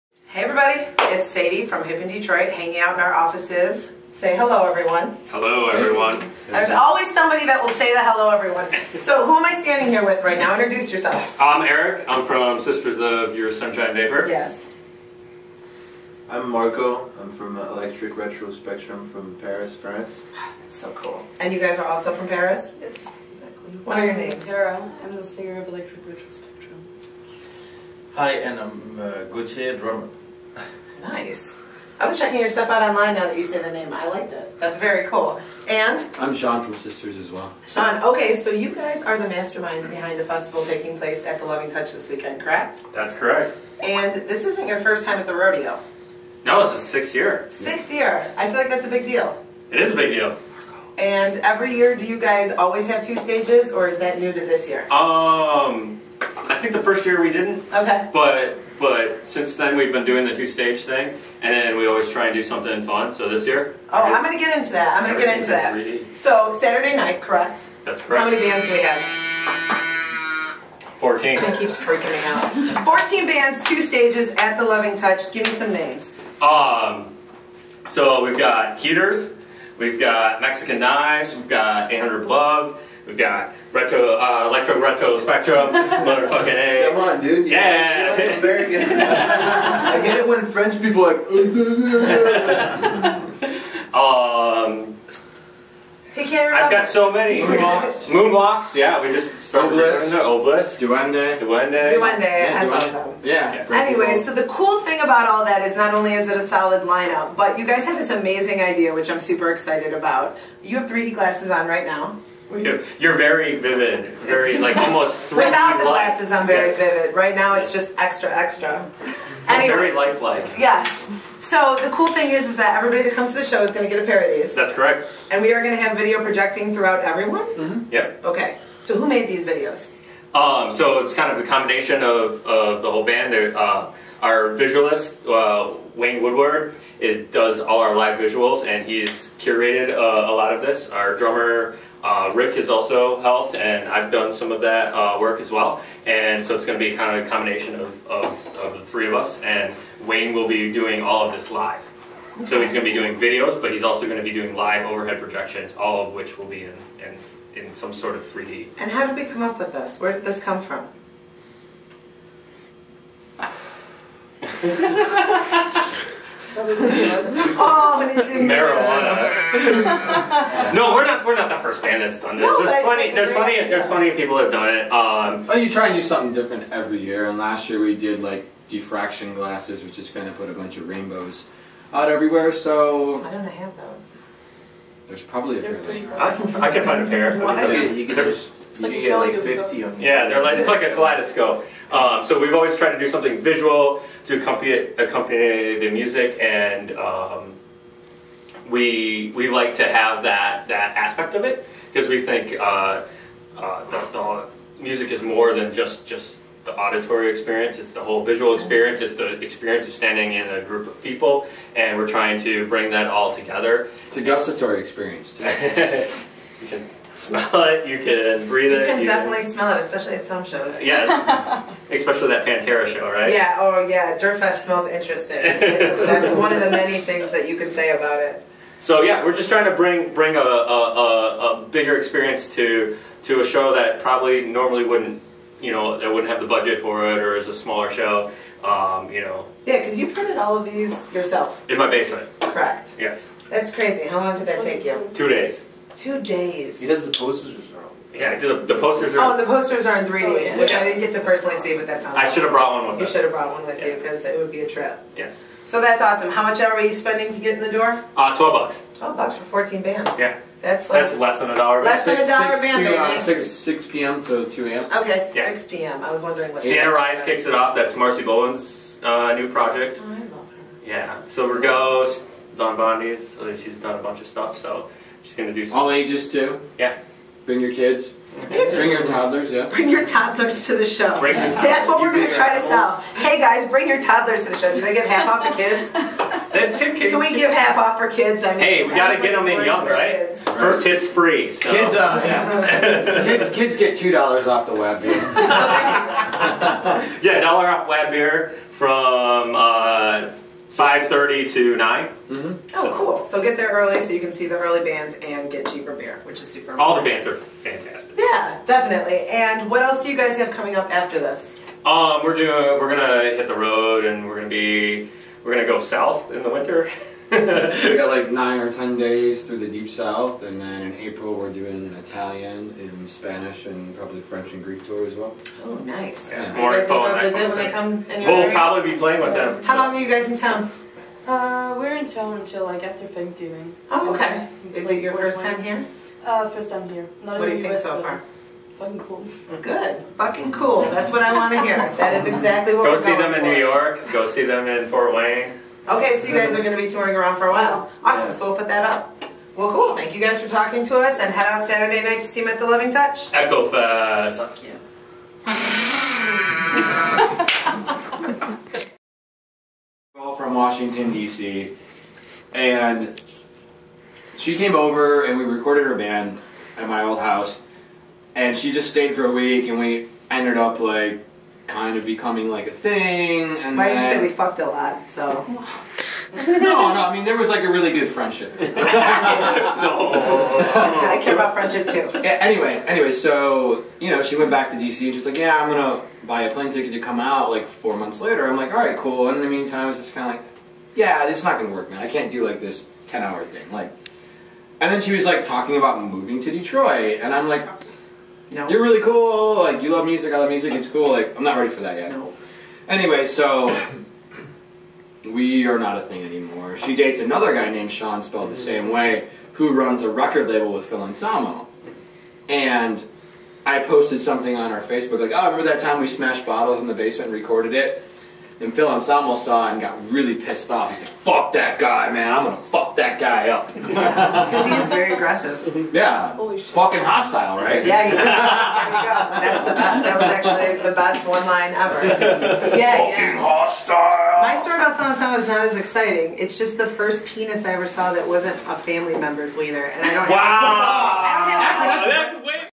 VLOG 10 w/ Sisters of Your Sunshine Vapor and Electro Retro Spectrum
VLOG 10 - Sisters of Your Sunshine Vapor and Electro Retro Spectrum talk ECHO Fest VI - for more info on the fest